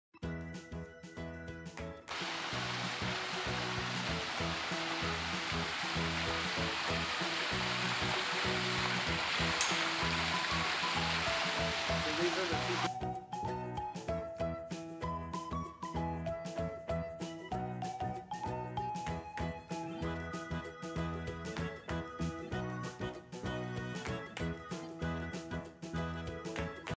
Ask the Pool Guy: Kyle Admires the FireBowls {Legendary Escapes}
Just what the title says... Kyle looks over the pool.  Handsome lad in my opinion.